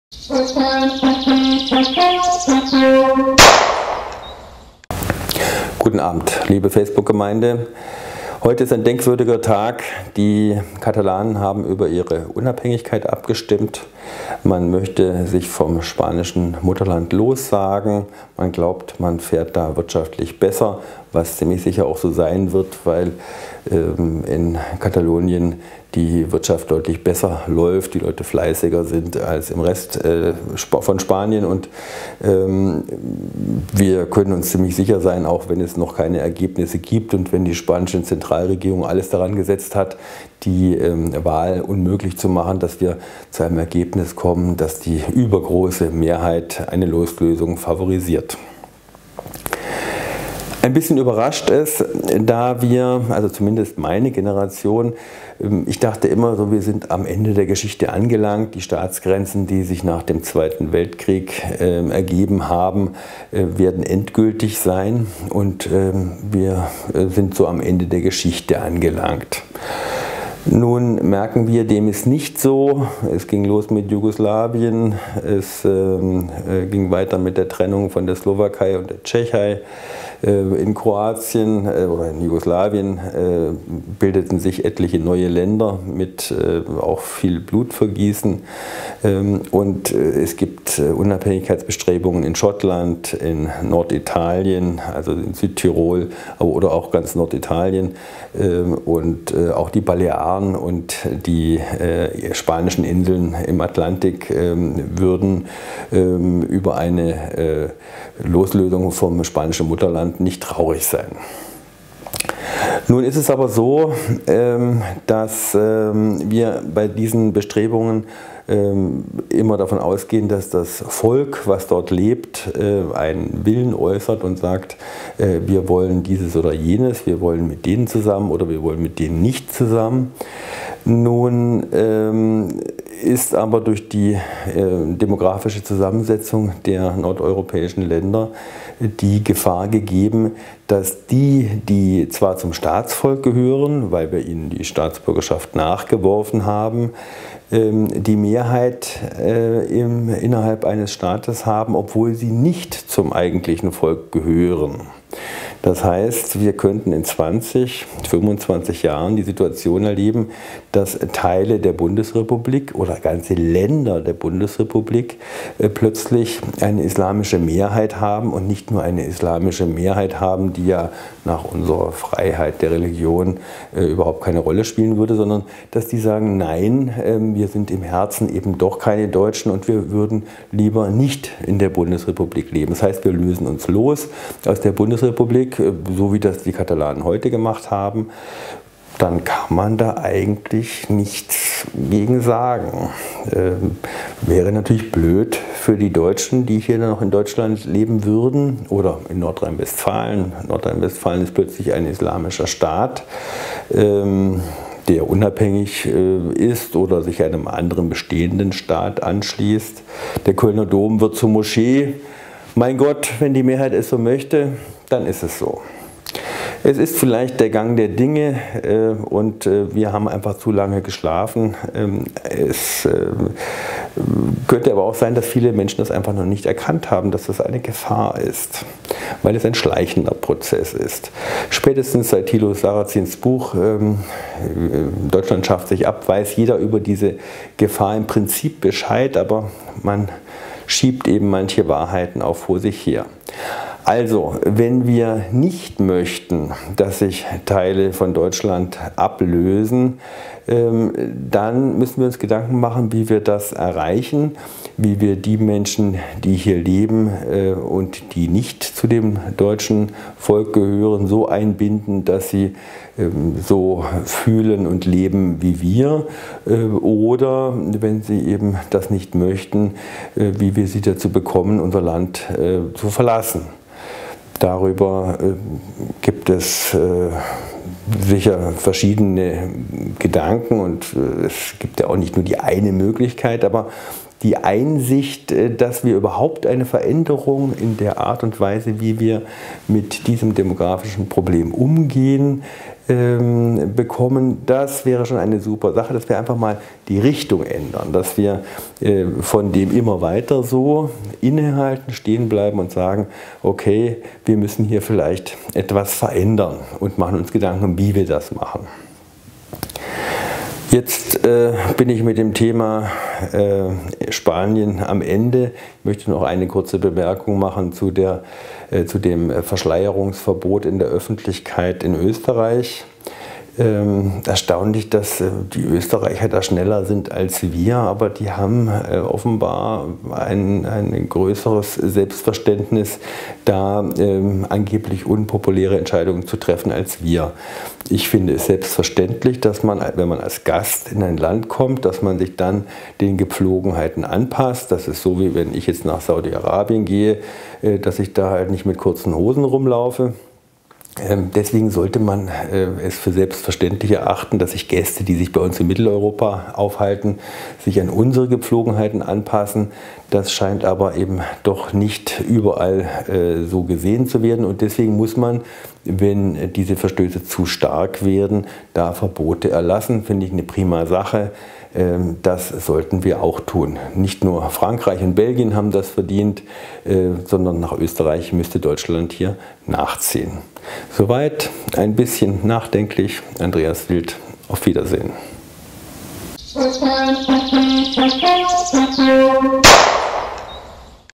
Katalonien stimmt ab - Konsequenzen für Europa
Loslösung von Landesteilen kann künftig auch aus anderen als wirtschaftlichen Gründen erfolgen.